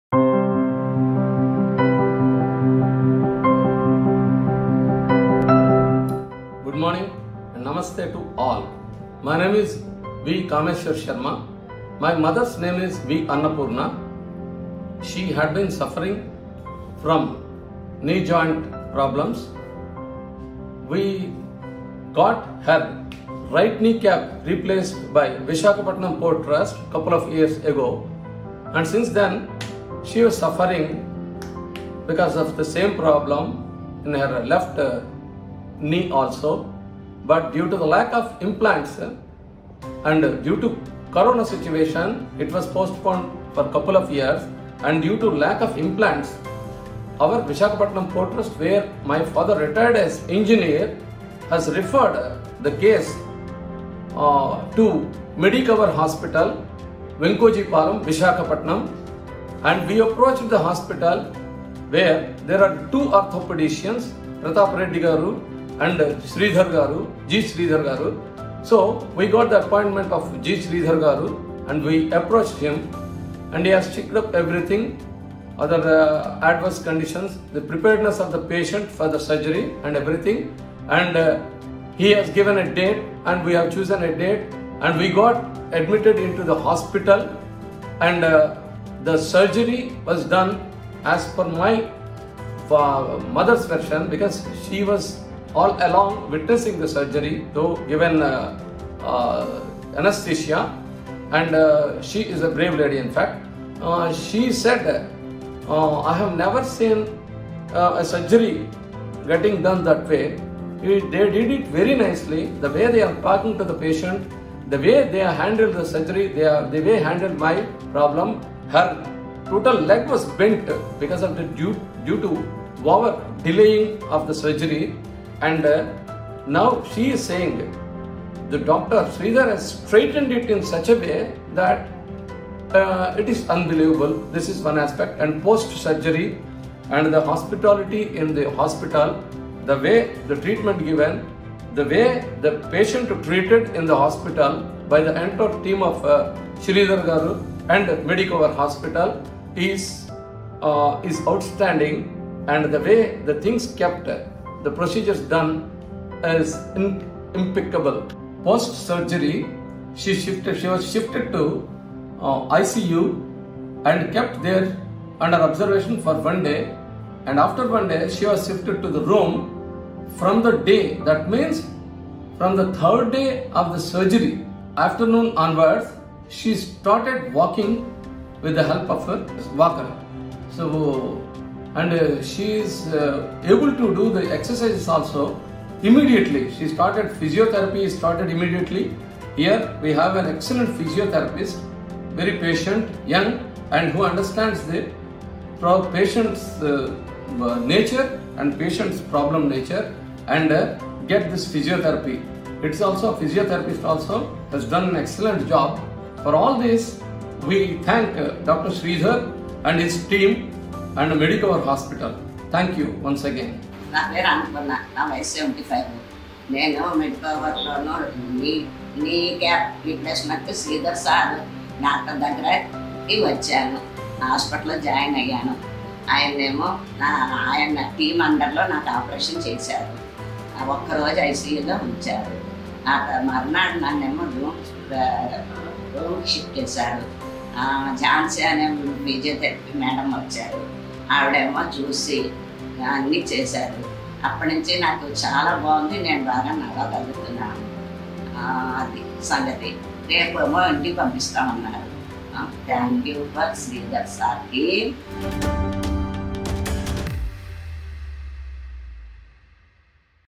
Total Knee Replacement | Patient Testimonial | Medicover Hospitals
“My name is V. Kameshwar Sharma, and my mother’s name is V. Annapoorna. She had been suffering from knee joint problems. We got her right knee cap replaced a couple of years ago. Since then, she has suffered from the same problem in her left knee. Due to lack of implants, our Visakhapatnam portals has referred the case to Medicover Hospitals. We approached the hospital, We got an appointment with Dr. Sridhar Gangavarapu, and he has checked up on everything. We got admitted into the hospital, and the surgery went well. The way they handled the surgery, the hospitality in the hospital, the way they treated the patient, and the whole team of Dr. Sridhar was outstanding. Post-surgery she got shifted to ICU and kept there under observation for one day. After one day, she got shifted to the room. From the third day of the surgery, she started walking with the help of her walker.” -  V. Kameshwar Sharma

He is very thankful for the support and care provided by the Hospital staff and showed his heartfelt gratitude to the Doctors.

We at Medicover Hospitals work round the clock and are always eager to serve you better, no matter how critical is the condition.